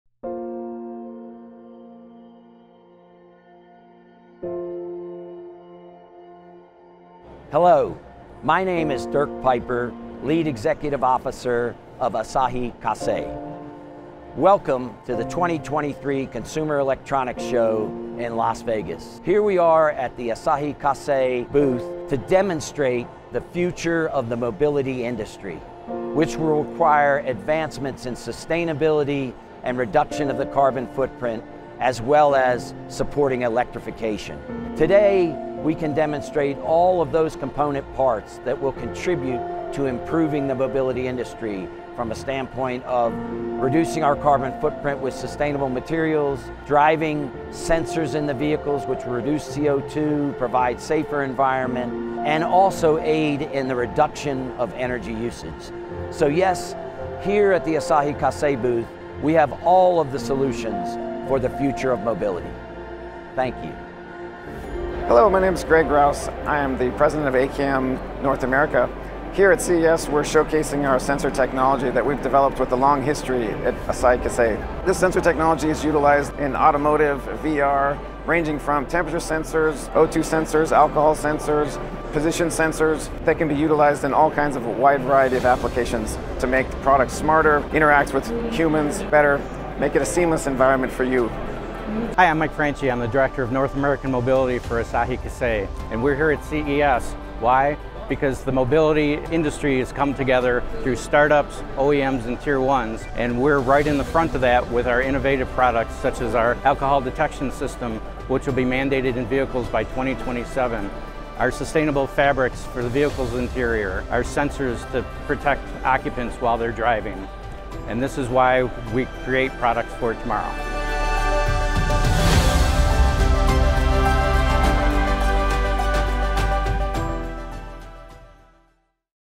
CES 2023 Digest_Future Vision of Mobility and Homes（Japanese subtitle） ―Asahi Kasei
旭化成グループは、2023年1月5日（木）～1月8日（日）に米国ラスベガスで開催された世界最大規模の技術見本市「CES® 2023」に出展しました。自動車やコンシューマ・エレクトロニクス業界関係者など約1,700名の方がブースを訪れました。